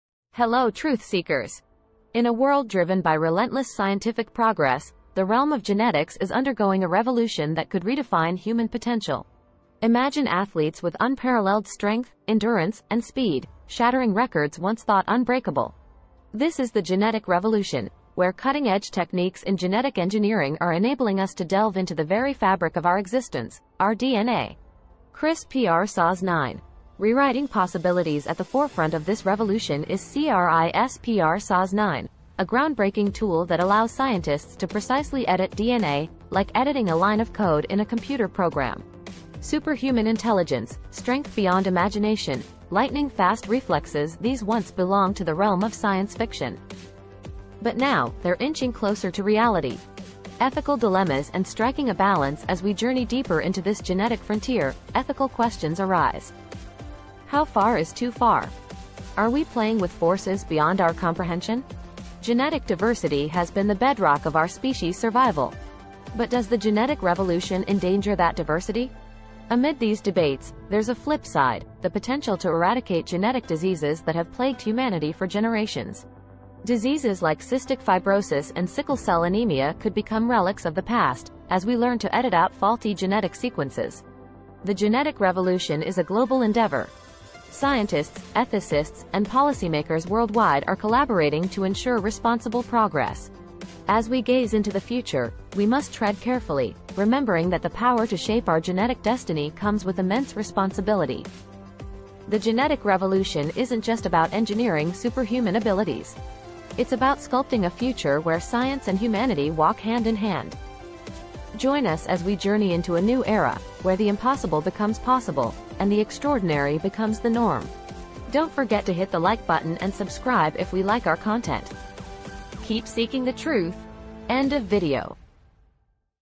The Genetic Revolution: Engineering Superhuman Abilities and Traits
Explore the cutting-edge realm of genetic engineering in our thought-provoking YouTube video, "The Genetic Revolution: Engineering Superhuman Abilities and Traits." Join us as we delve into the world of CRISPR-Cas9 technology and its potential to rewrite human DNA, paving the way for superhuman attributes and extraordinary feats. From unprecedented athleticism to unparalleled intelligence, witness the exciting possibilities that lie ahead.
But with great power comes great responsibility. As we unlock the potential to sculpt our genetic destiny, ethical dilemmas arise. Join the conversation about the delicate balance between progress and ethical considerations, and discover the collaborative efforts of scientists, ethicists, and policymakers to guide us forward.
This video doesn't just focus on engineering superhuman abilities; it illuminates the broader impact on genetic diversity, potential cures for genetic diseases, and the responsible navigation of this genetic frontier. Embark on a journey into a future where science and humanity harmonize, and where the extraordinary becomes the new norm.